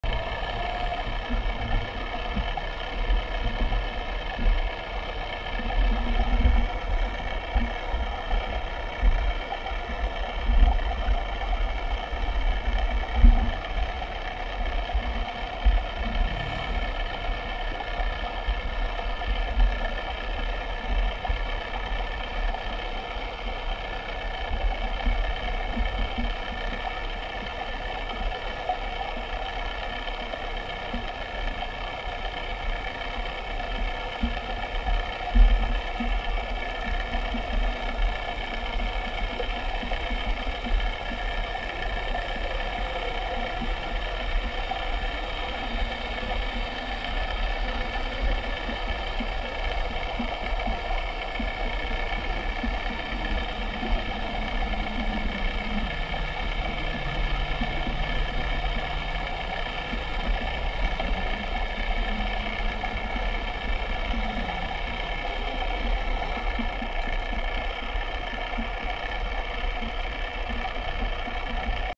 Clydagh boat 1280x720
A 35ft vessel, probably iron age, with intact transom, steering oar, quarter posts and gunwales.